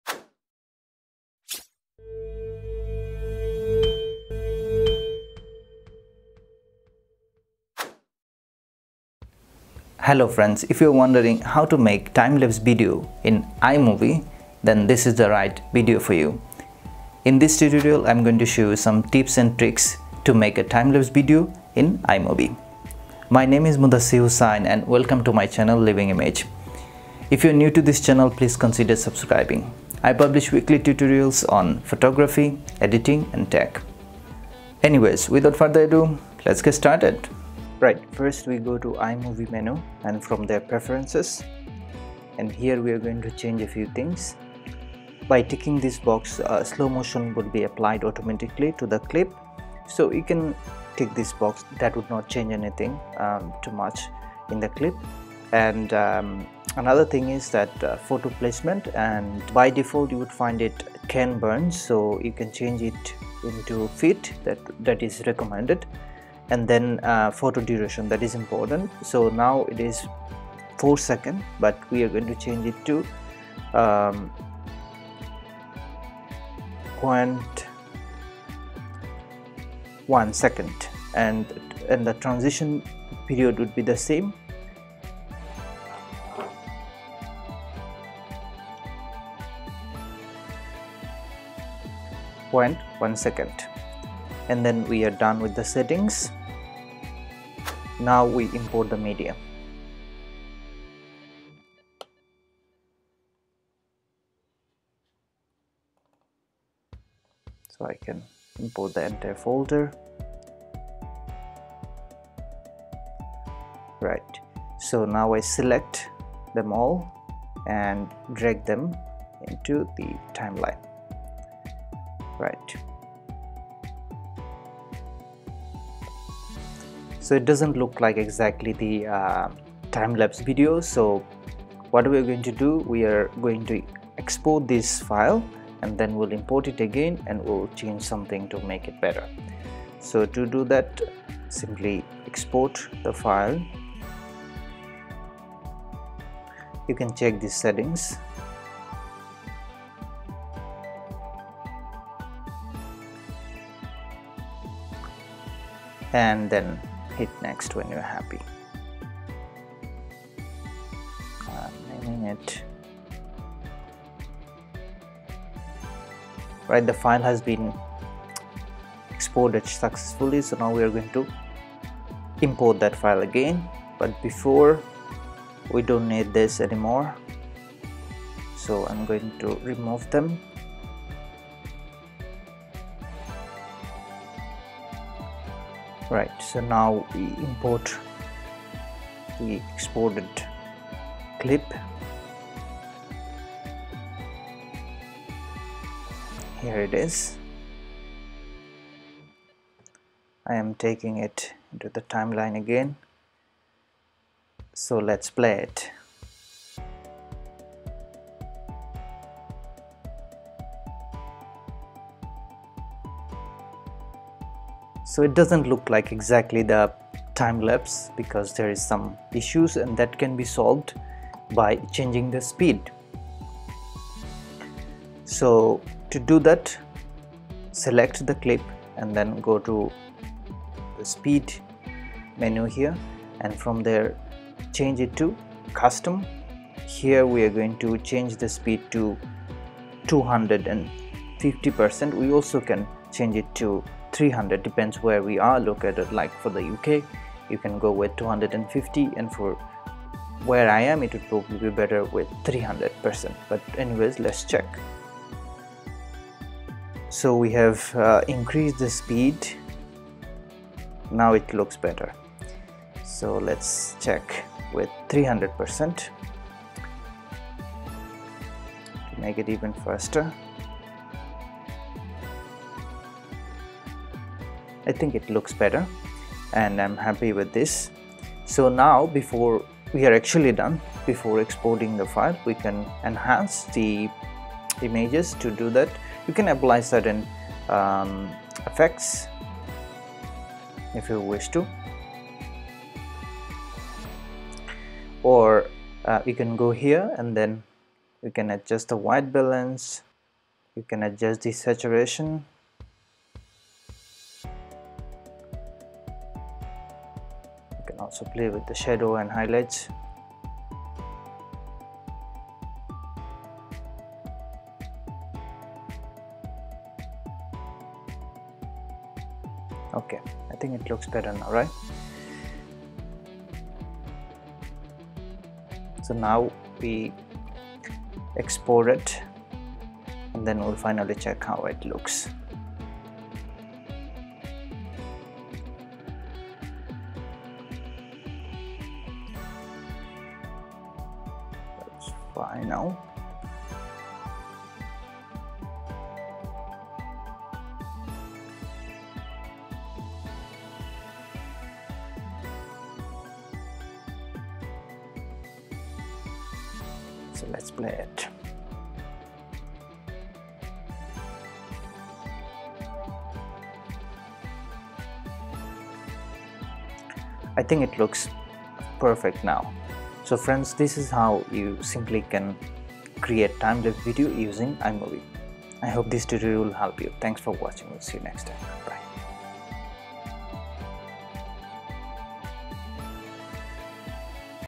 How to Create Timelapse Video with iMovie | Make a Timelapse Video from Still Images
Hello friends,

In this video, I am going to show you a few trips and tricks to create a timelapse video from still images using iMovie.

Thanks
LI